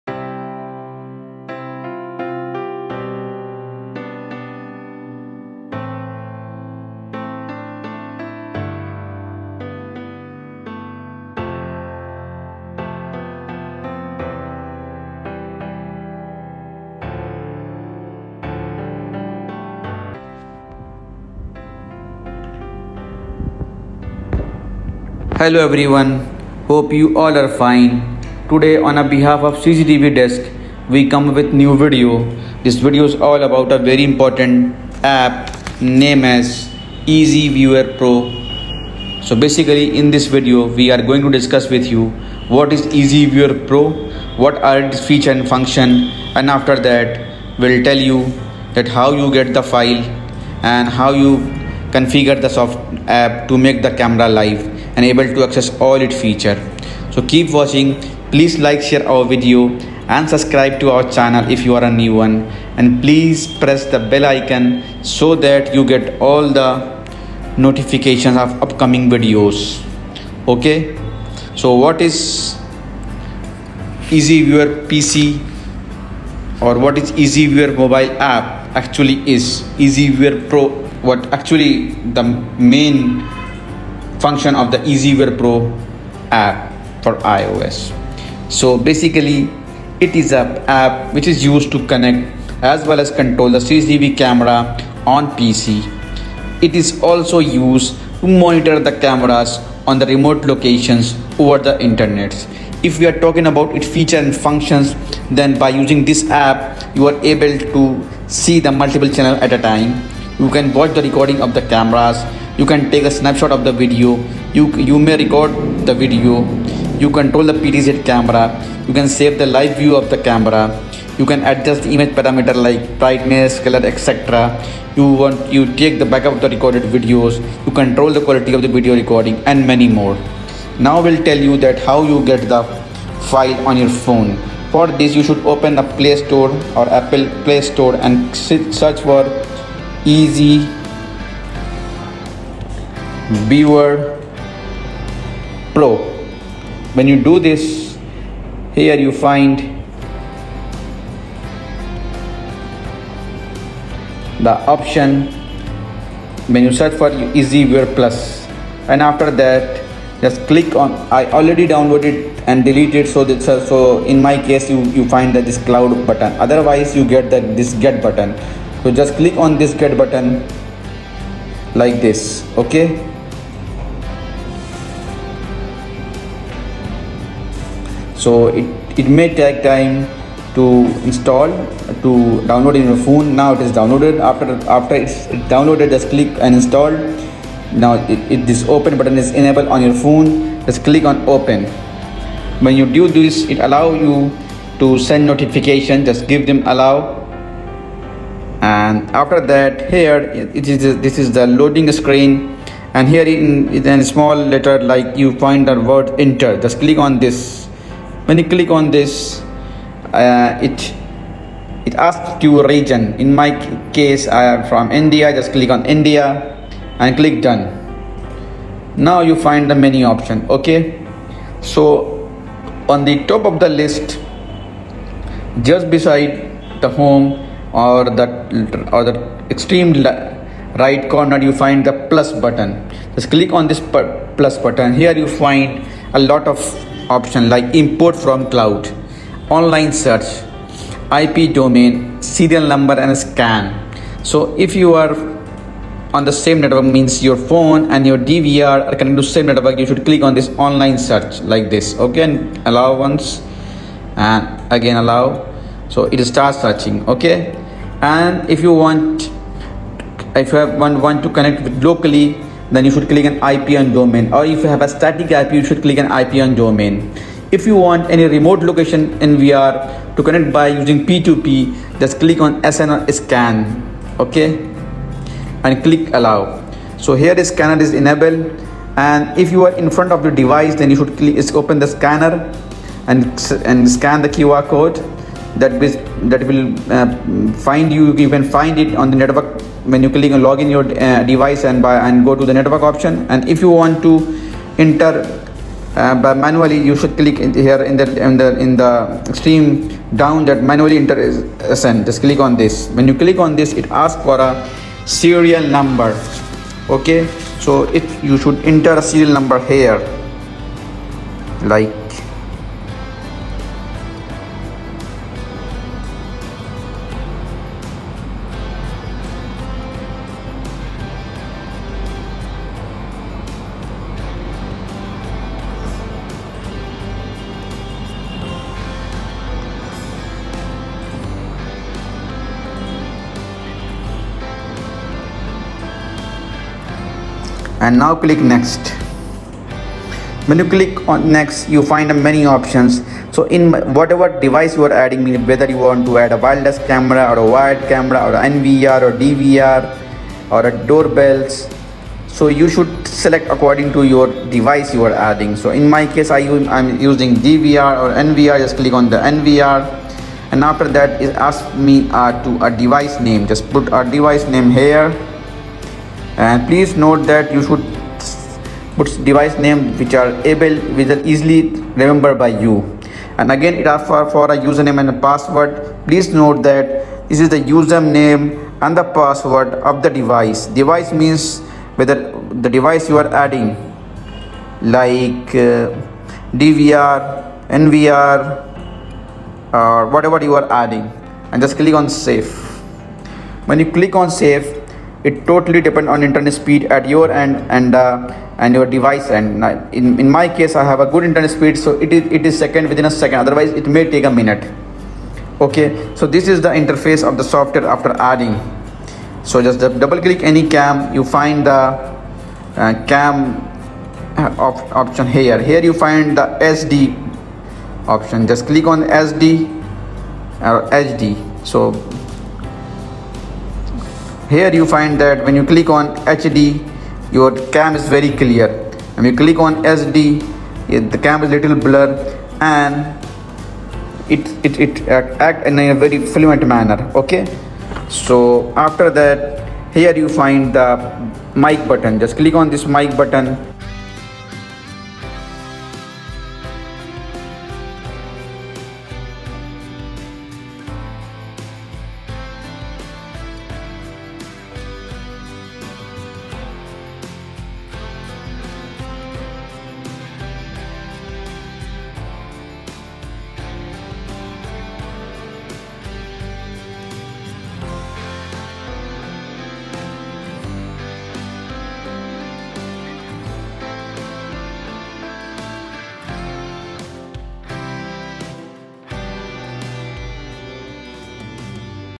How to Install EasyViewer Pro for iPhone App & Configure it for Monitoring?
This EasyViewer Pro App is for security surveillance. This video explains to you the complete installation and configuration process for EasyViewer Pro for iPhone. The logging to the app and the device addition steps are demonstrated with full illustration and narration. This software is designed, developed and offered by OVERSEAS. It has already more than 9 Million subscribers around the globe. This mammoth subscription scale tells volumes about the EasyViewer Pro App. The EasyViewer Pro App configuration is easy. When you configure it, you enable yourself to watch your locations from any remote area. This App EasyViewer Pro supports viewing and controlling live video streams from cameras and video encoders. You can handle the device from your location. You can pan the CCTV camera, tilt it and zoom it manually from your location, no matter how far you are from the site. You can communicate two-way with the help of EasyViewer Pro for iPhone App. The person on the device end can receive your message and reply to you. This feature helps in communicating messages or alerting people of threats. The powerful sensors push messages and howl alarms sensing any suspicious activity or person. This EasyViewer Pro App is remarkably useful in safeguarding your security. This video gives you at length the installation and configuration processes. The EasyViewer Pro App set-up is demonstrated in a very lucid way. The narration explains every step. Watch the complete video and enjoy the proceedings.